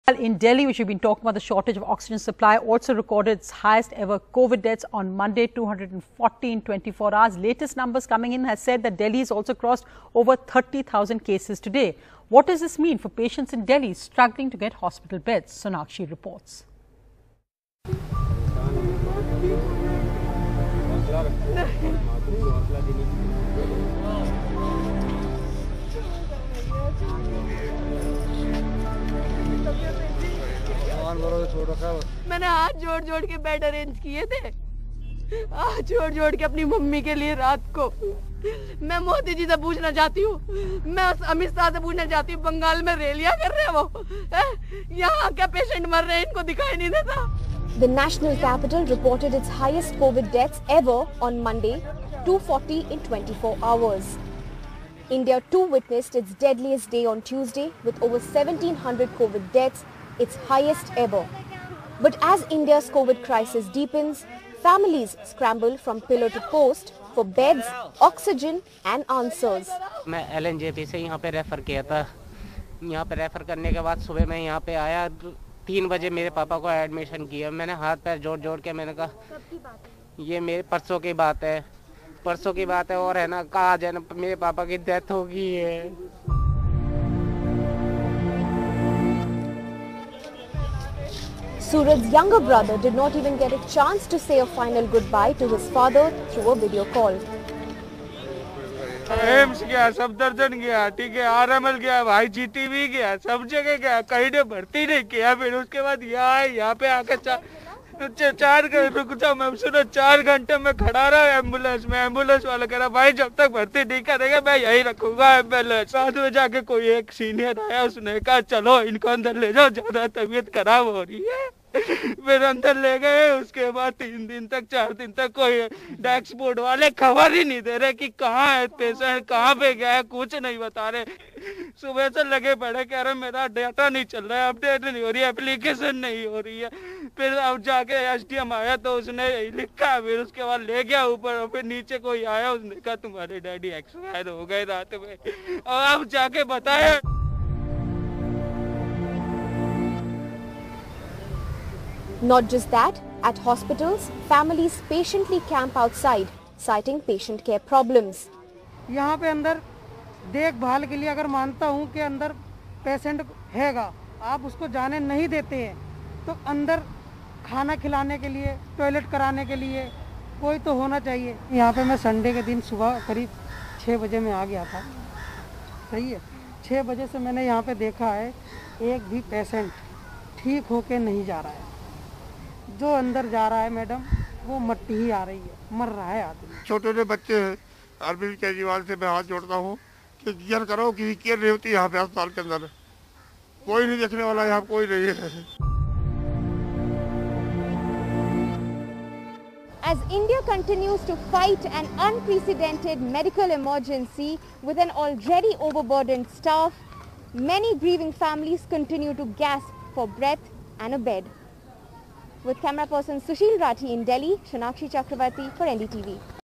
Record Deaths In Delhi: The Human Tragedy
Delhi Covid-19 News: As India's capital reports its highest COVID-19 deaths ever on Monday - 240 deaths in 24 hours, heart-wrenching stories of people and their struggles as families of COVID-19 patients scramble for beds, oxygen having to say their final goodbyes over video call.